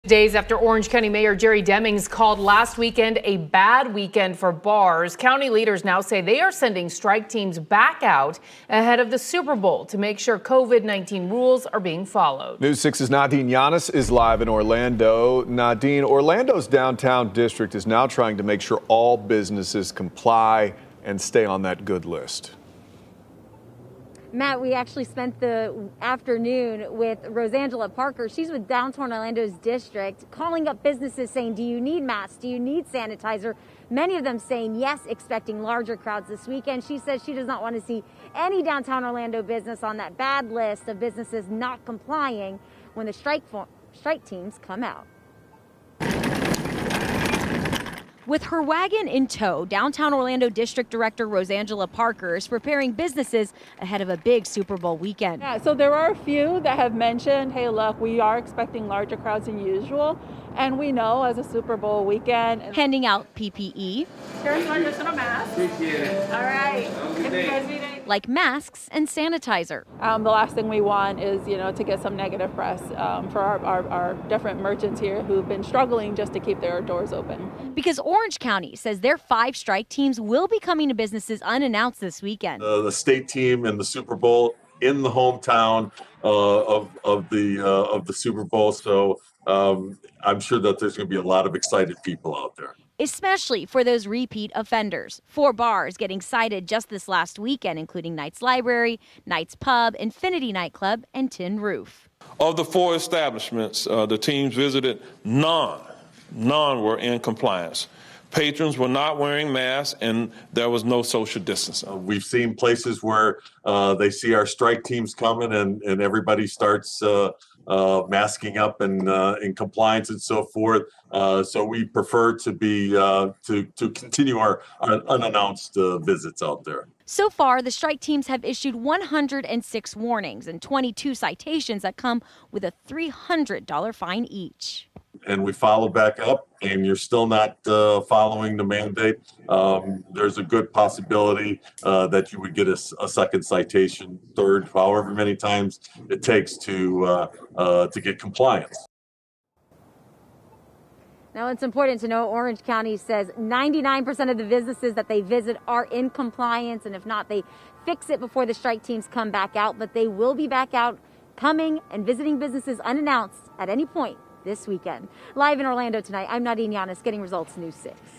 ‘We will be out:’ Orange County strike teams prepare for big Super Bowl weekend
Orange County’s five strike teams will be out visiting businesses unannounced this weekend ahead of the Super Bowl.